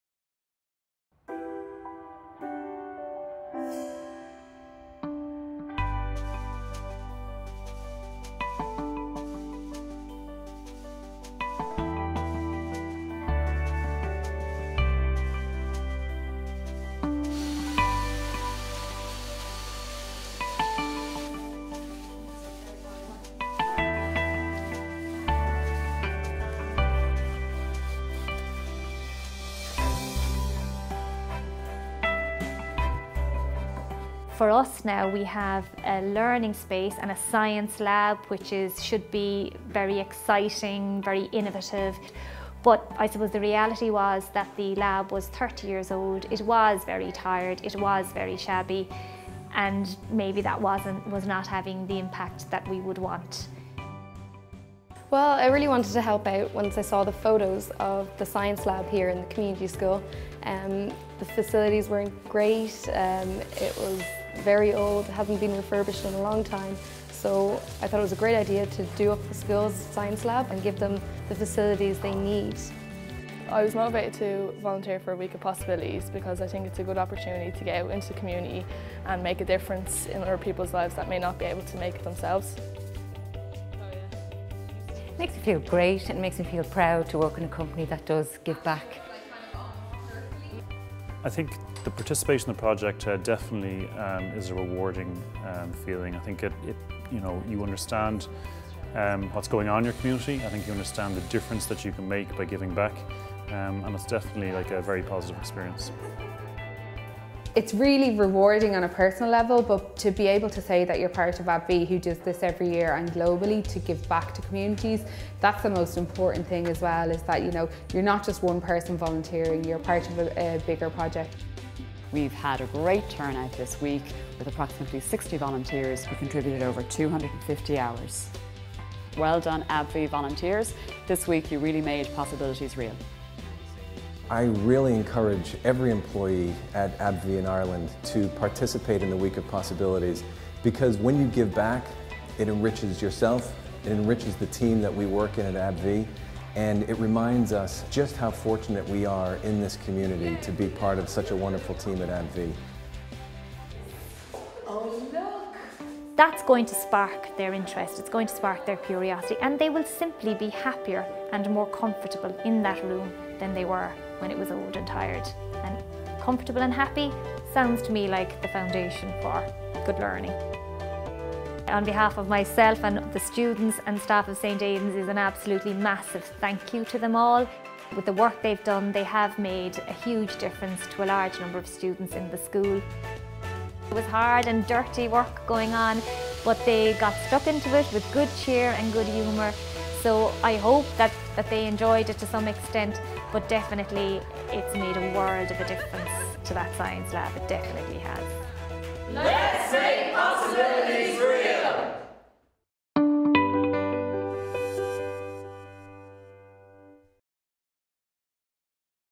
Abbvie Week Of Possibilities in Dublin, Ireland 2017
AbbVie recently celebrated its fourth annual Week of Possibilities. The week-long initiative is AbbVie’s signature annual volunteering project and more than 6,000 employees in 50+ countries come together to make a difference in their respective communities around the world.

Almost 60 AbbVie employees from AbbVie’s commercial offices in Citywest completed the extensive refurbishment of a science lab in St Aidan’s Community School in Tallaght during the week-long initiative. The activity reflects the company’s ongoing commitment to science education, and means that students will have a stimulating and fit-for-purpose science facility when they return from their summer recess.

This video was produced, directed, filmed and edited by Mark Flagler / Flagler Films. Professional photographs captured by Rob O'Connor (Rocshot).